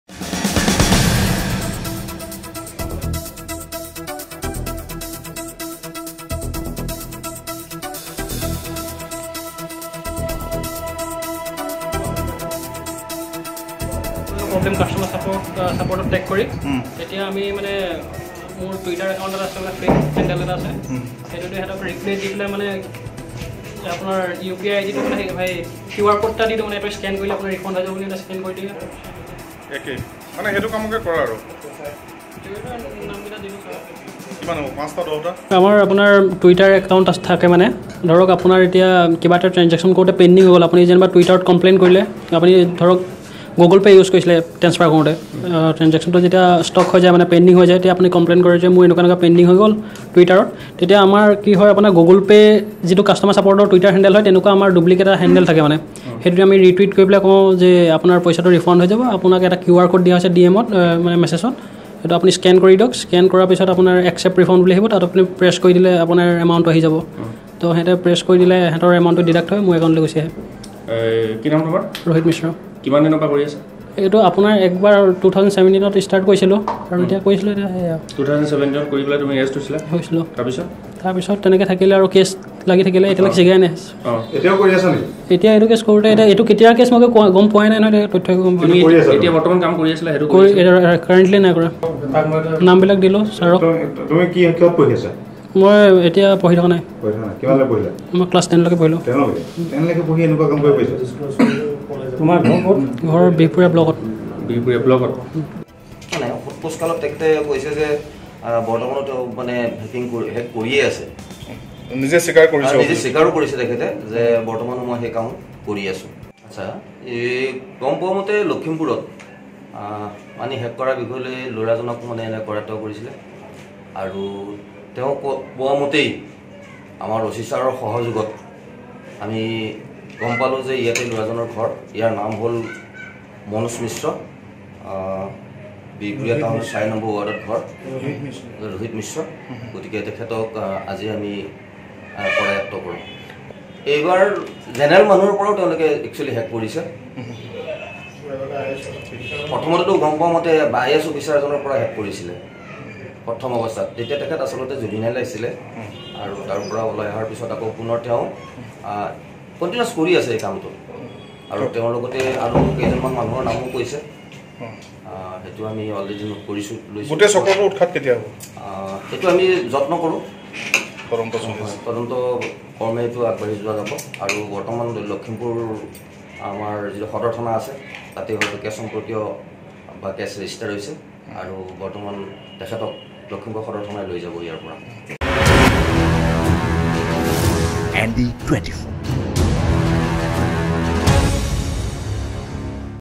লখিমপুৰত আৰক্ষীৰ জালত দুই অনলাইন হেকাৰ
আপোনাৰ সাঁচতীয়া ধনত হেকাৰৰ চকু
দিল্লীৰ ব্যক্তিৰ গোচৰৰ ভিত্তিত গ্ৰেপ্তাৰ দুই হেকাৰক
বিহপুৰীয়া আৰক্ষীৰ অভিযানত গ্ৰেপ্তাৰ ৰোহিত মিশ্ৰক
অনলাইনেৰে ধন হৰলুকীৰ মাষ্টাৰমাইণ্ড ৰোহিত মিশ্ৰ
তিনি বছৰ পূৰ্বেও একে অপৰাধত গ্ৰেপ্তাৰ হৈছিল ৰোহিত